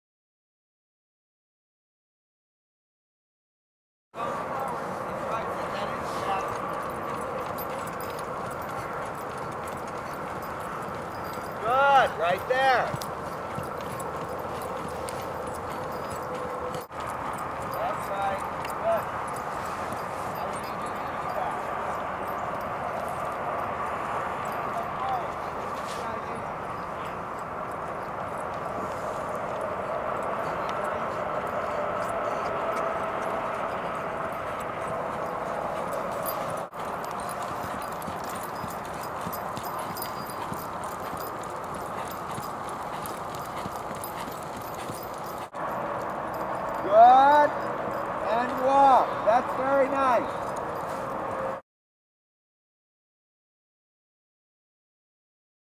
Hoop Dance
Nationals quality 6 year old Half Arabian Country Pleasure/Halter/Driving mare.  Safe, sane, sporty, and fun!!!  She already has numerous National, Regional, and Class A titles in Halter and Country Pleasure. Added Show Hack and Driving to her repertoire this year.  Awesome all around show horse for youth or amateur!  This video is from late September 2012 and shows clips of a harness schooling session with Ray LaCroix.